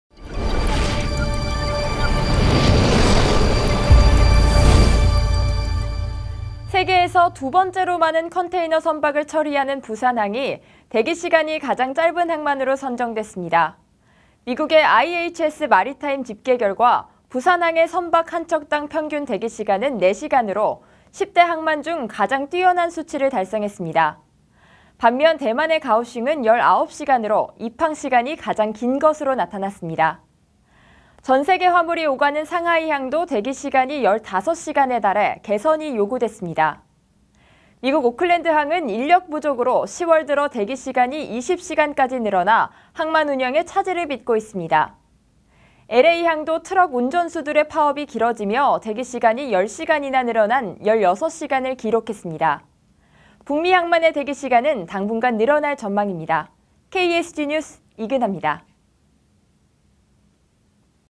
부산항 대기시간 最短 항만 '타이틀'
세계에서 두 번째로 많은 컨테이너 선박을 처리하는 부산항이 대기시간이 가장 짧은 항만으로 선정됐습니다.

미국의 IHS마리타임 집계 결과 부산항의 선박 한 척당 평균 대기시간은 4시간으로 10대 항만 중 가장 뛰어난 수치를 달성했습니다.

반면 대만의 가오슝은 19시간으로 입항시간이 가장 긴 것으로 나타났습니다. 전 세계 화물이 오가는 상하이항도 대기시간이 15시간에 달해 개선이 요구됐습니다.

미국 오클랜드항은 인력 부족으로 10월 들어 대기시간이 20시간까지 늘어나 항만 운영에 차질을 빚고 있습니다. LA항도 트럭 운전수들의 파업이 길어지며 대기시간이 10시간이나 늘어난 16시간을 기록했습니다.

북미 항만의 대기시간은 당분간 늘어날 전망입니다.